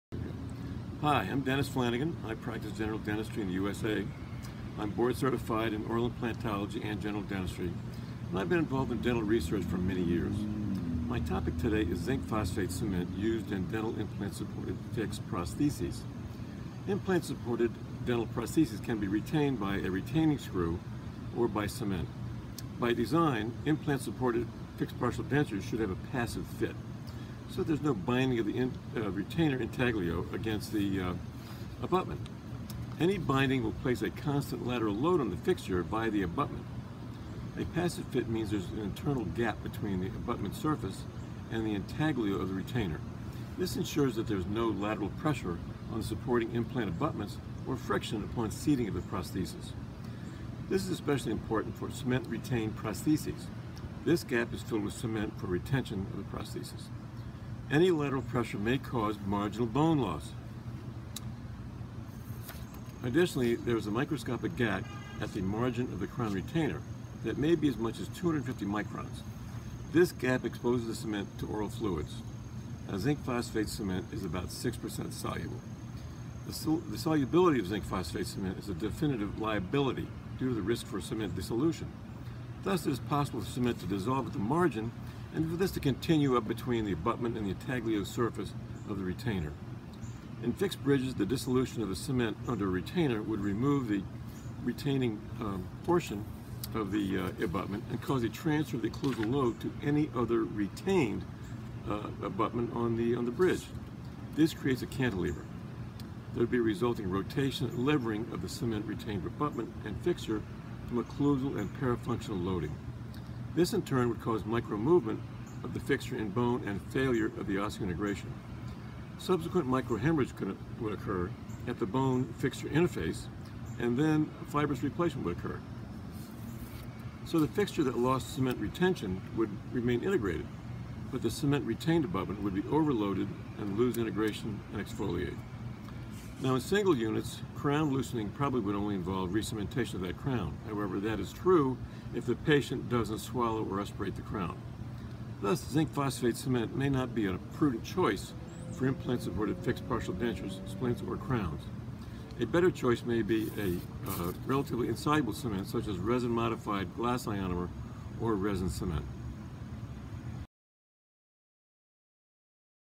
Zinc phosphate as a definitive cement from PDF - Video Abstract ID 146544
Video abstract of case series “Zinc phosphate as a definitive cement for implant-supported crowns and fixed dentures” published in the open access journal Clinical, Cosmetic and Investigational Dentistry by Flanagan.

Abstract: Implant-supported dental prostheses can be retained by a screw or cement. Implantsupported fixed partial dentures have a passive fit. A passive fit means there is an internal gap between the abutment surface and the intaglio of the retainer to insure that there is no lateral pressure on the supporting implants or friction upon seating of the prosthesis. This gap is filled with cement for retention of the prosthesis. Any lateral pressure may cause marginal bone loss or periimplantitis. Also, there is usually a microscopic gap at the margin of a crown retainer that exposes the cement to oral fluids. The solubility of zinc phosphate (ZOP) cement is a definite liability due to the risk for cement dissolution. In fixed prostheses, the dissolution of the cement of one or more retainers would cause a transfer of the occlusal load to the retained unit(s). The resulting rotation and lifting of the cement-retained implants from occlusal and parafunctional loads could cause loss of osseointegration of the abutment-retained implant(s). ZOP cement may not be indicated for implant-supported fixed partial dentures or splints. Cement dissolution in single unit probably only involves re-cementation, if the patient does not swallow or aspirate the crown.